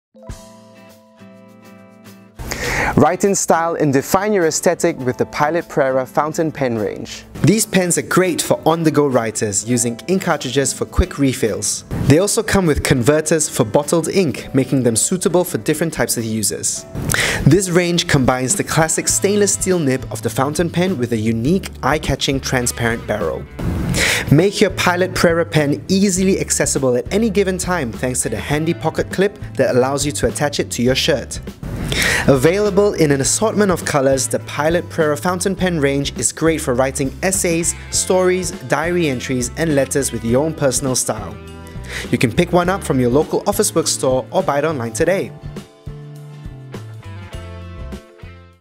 Pilot Prera Fountain Pen Overview
The Pilot Prera Fountain Pen is a great pen for anyone who wants to write with a traditional style pen. The pen uses ink cartridges but also comes with a converter if you prefer using bottled ink.